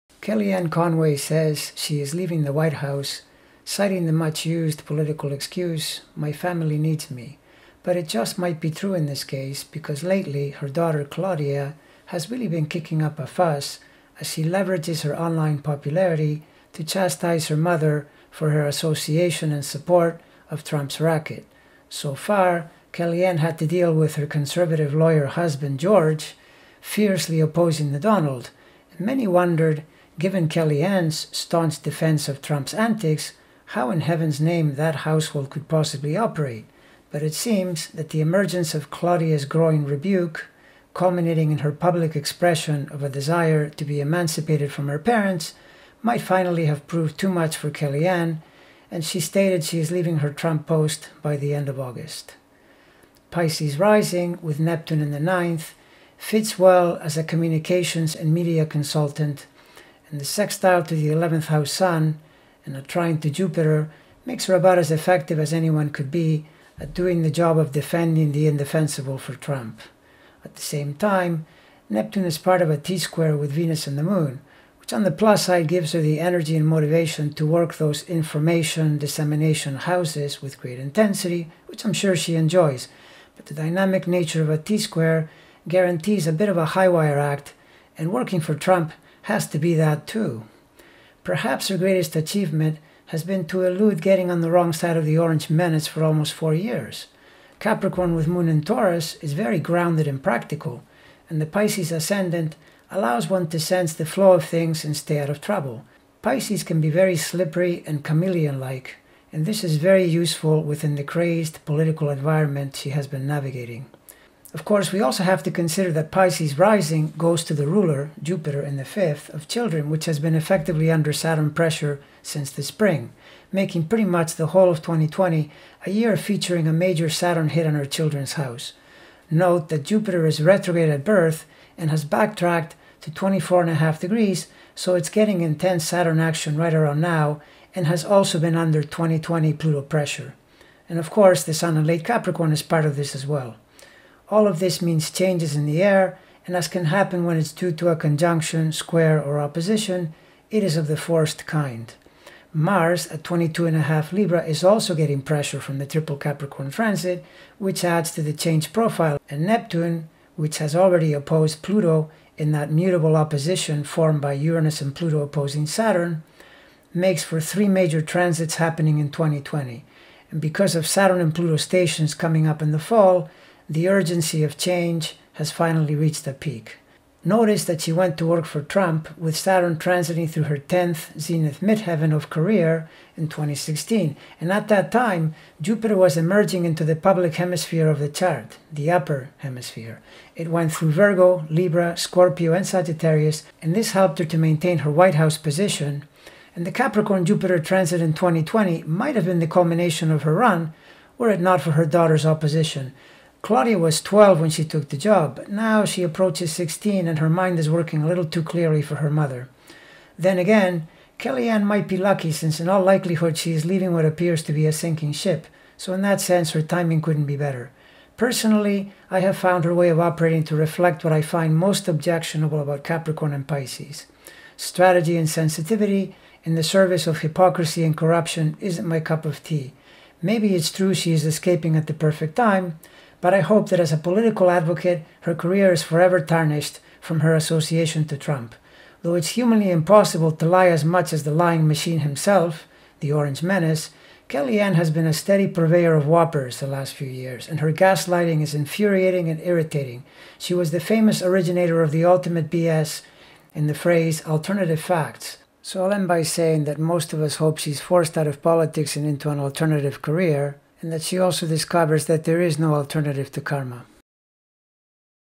Kellyanne Conway Leaves the Whitehouse--Astrology
In response to trouble in the home sphere,  Kellyanne Conway, a key Trump ally and advisor since 2016, will be leaving her Whitehouse job by the end of this month. Increasing trouble from her 16 year old daughter-Claudia, appears to be the main reason. Her husband, George Conway, a fierce Trump detractor,  is also stepping back from his work at the Lincoln Project.